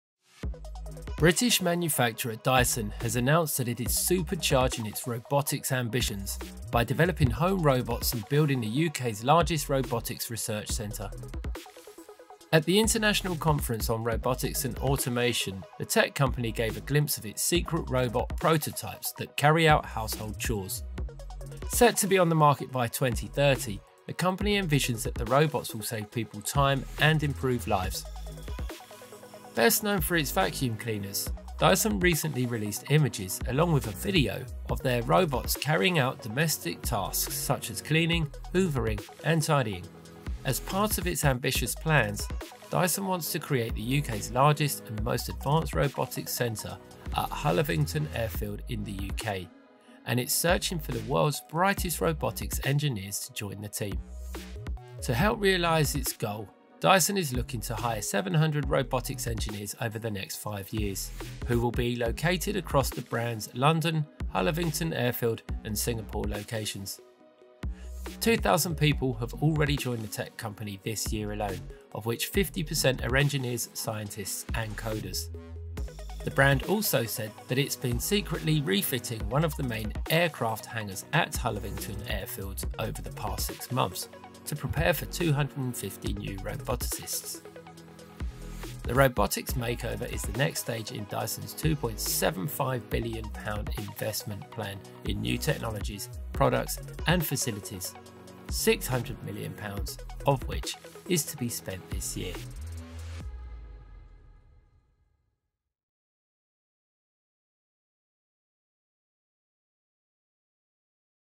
Dyson reveals home robots that can carry out domestic chores
British manufacturer Dyson has announced that it is "supercharging its robotics ambitions" by developing home robots that can carry out domestic chores as well as investing building the UK's largest robotics research centre.

At the International Conference on Robotics and Automation (ICRA) in Philadelphia, the tech company gave a glimpse of its secret robot prototypes that carry out household chores. Set to be on the market by 2030, the company envisions that the robots will save people time and improve lives.

best known for its vacuum cleaners, Dyson recently released images, along with a video, of its robots carrying out domestic tasks such as cleaning, hoovering and tidying.

And as part of its ambitious plans, Dyson wants to create the UK's largest and most advanced robotics center at Hullavington Airfield, and it's searching for the world's brightest robotics engineers to join the team

To help realize its goals, Dyson is looking to hire 700 robotics engineers over the next five years, who will be located across the brand’s London, Hullavington Airfield, and Singapore locations. 2,000 people have already joined the tech company this year alone, of which 50% are engineers, scientists, and coders. The brand also said that it’s been secretly refitting one of the main aircraft hangars at Hullavington Airfield over the past six months to prepare for 250 new roboticists. The robotics makeover is the next stage in Dyson’s £2.75bn investment plan in new technologies, products and facilities; £600m of which is to be spent this year.